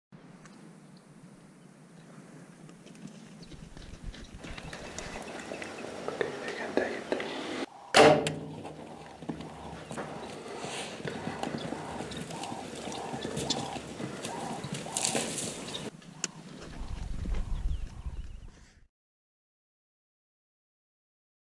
Alan Harris Buffalo Shot
Have a look at Alan's Buffalo Shot from one of our new Pit blinds. Some exciting stuff!